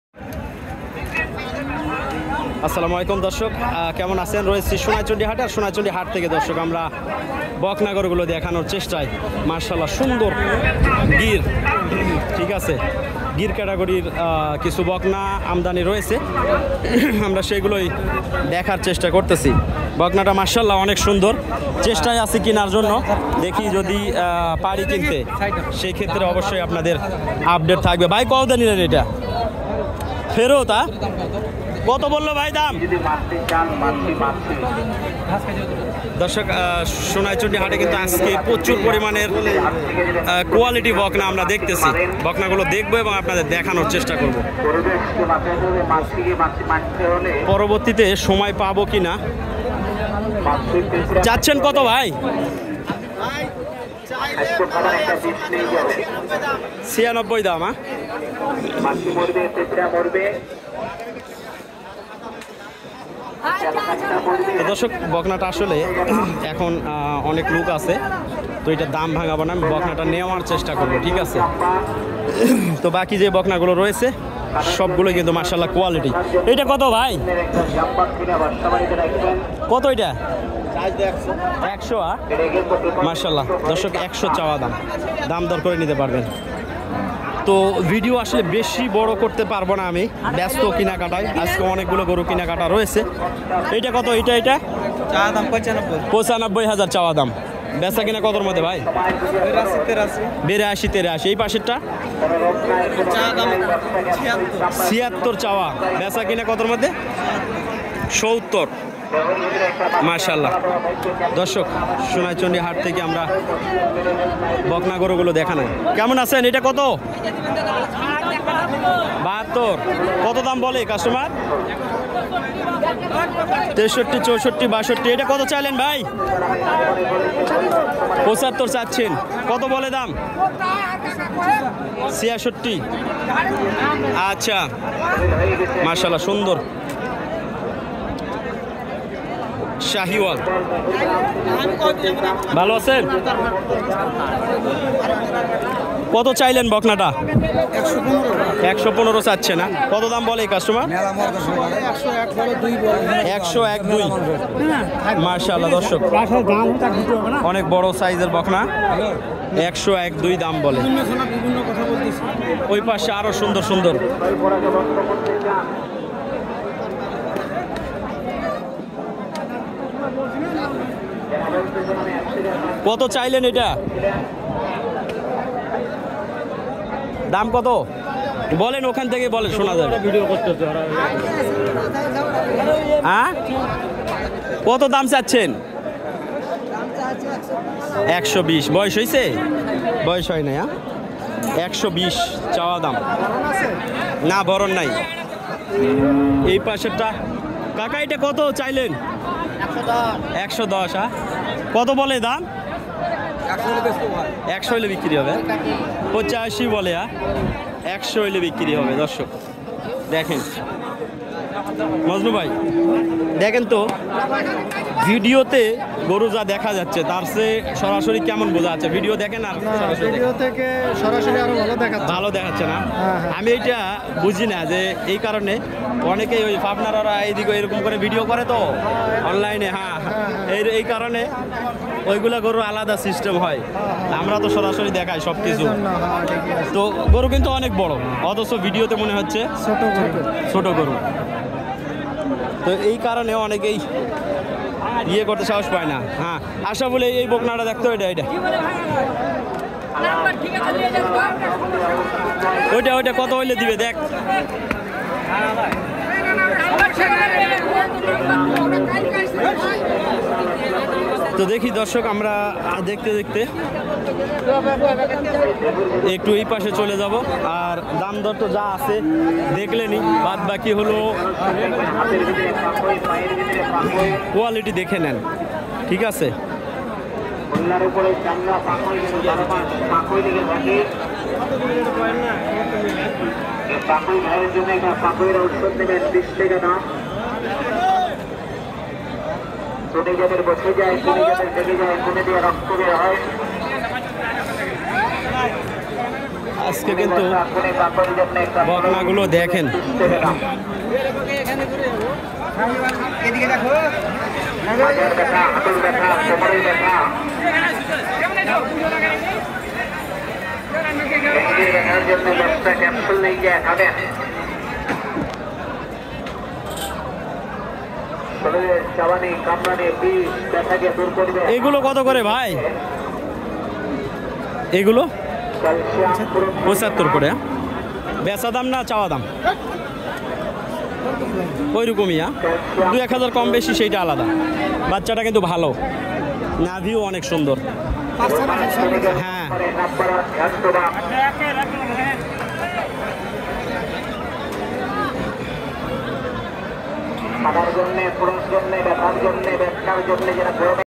আজ ২৯ মে ২০২৪ || সব গুলোই হাই কোয়ালিটি বকনা || শাহিওয়াল, গীর || সোনাইচন্ডী হাট || চাপাই জেলা
এই হাট বসে সপ্তাহে একবার বুধবার